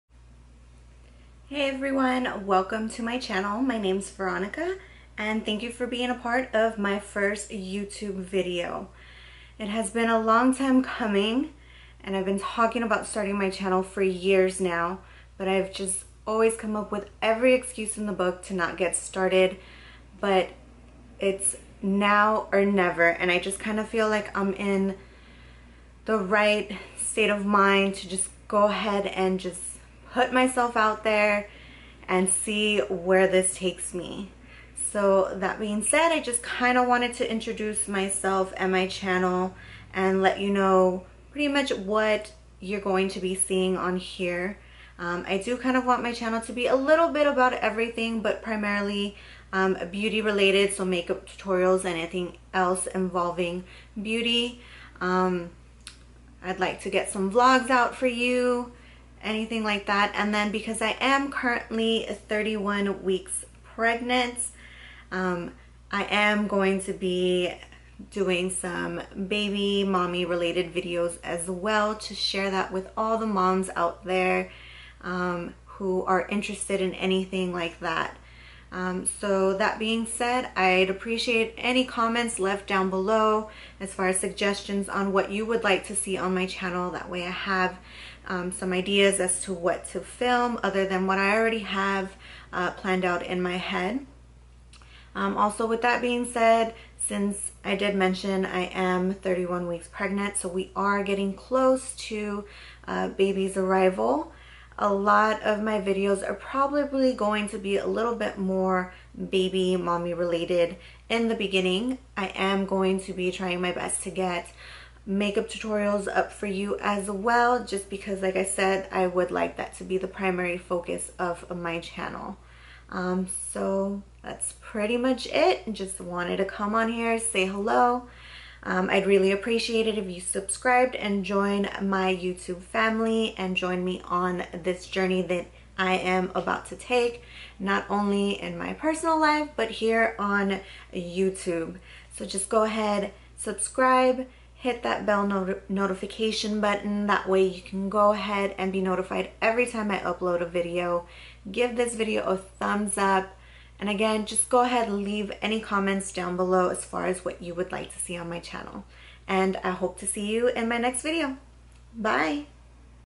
Hello YouTube!
Hello YouTube! I just wanted to come and say hello and introduce myself along with my channel. In the future I will be posting makeup tutorials and other beauty related content, along with Vlogs, baby/mommy related videos and any other suggestions you all might have. I would love for you to join my YouTube family and be a part of my journey here. I hope to see you all in my next video 😘